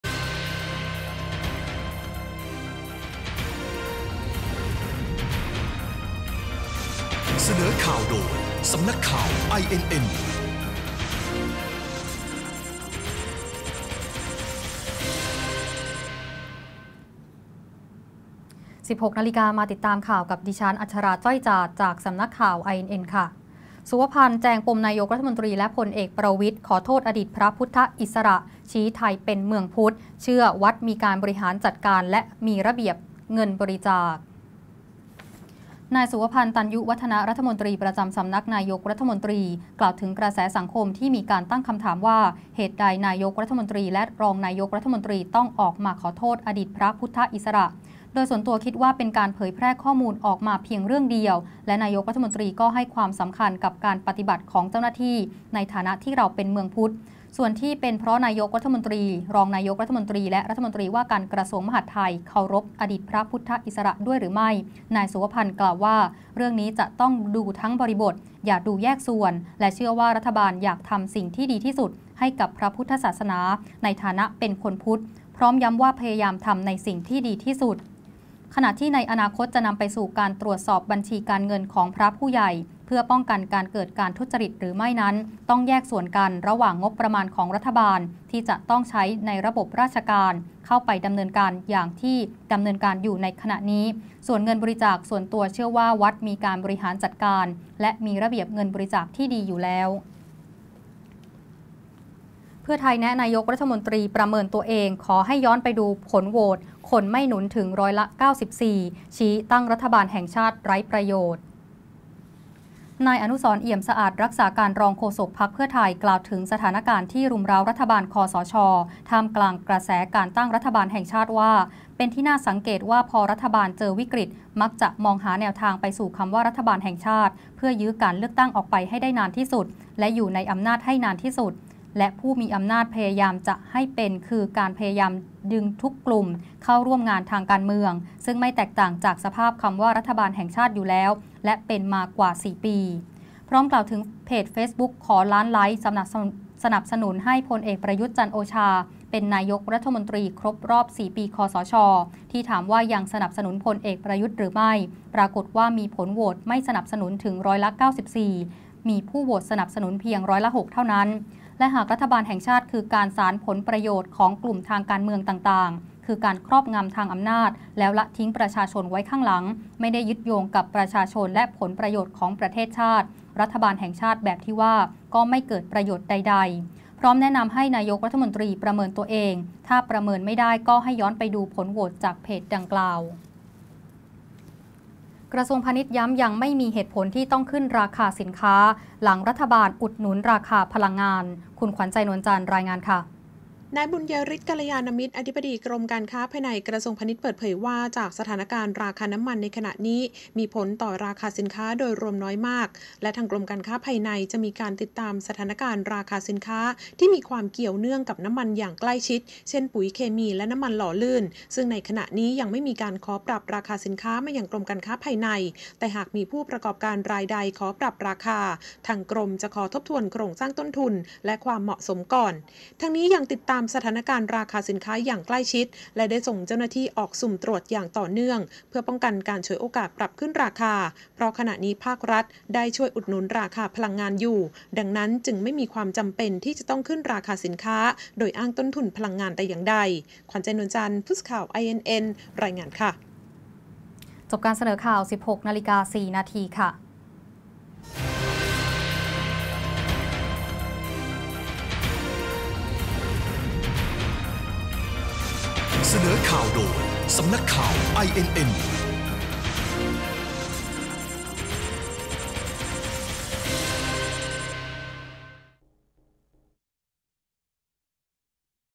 ข่าวต้นชั่วโมง 16.00 น.(27/05/61)
-"สุวพันธุ์"แจงปมนายกฯขอโทษ"อดีตพระพุทธอิสระ"
-พท.แนะนายกฯประเมินตัวเองขอให้ย้อนไปดูผลโหวต
-พณ.ย้ำไม่มีเหตุผลขึ้นราคาสินค้า